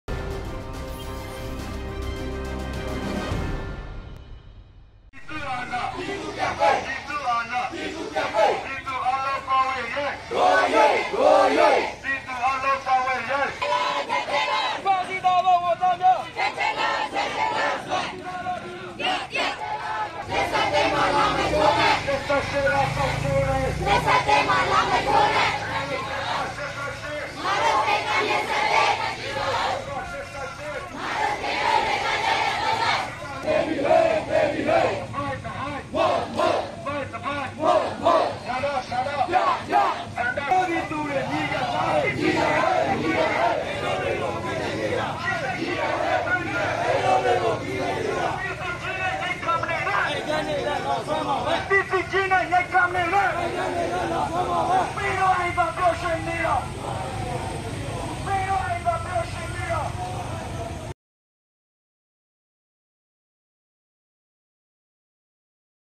သရက်ချောင်းမြို့နယ် ကနက်သီရိရွာတွင် စစ်အာဏာရှင်ကိုဆန္ဒပြ
တနင်္သာရီတိုင်းဒေသကြီး၊ သရက်ချောင်းမြို့နယ်၊ ကနက်သီရိကျေးရွာတွင် စစ်အာဏာရှင်စနစ်ဆန့်ကျင်ရေး၊ ဖမ်းဆီးထားသူများအားပြန်လွှတ်ပေးရေး၊ ဒီမိုကရေစီပြန်လည်ရှင်သန်လာစေရေးတို့အတွက် ကျေးရွာသူ/သားများမှ ယနေ့ မေလ (၁၄)ရက်နေ့တွင်သပိတ်မှောက်ဆန္ဒပြခဲ့ကြပါသည်။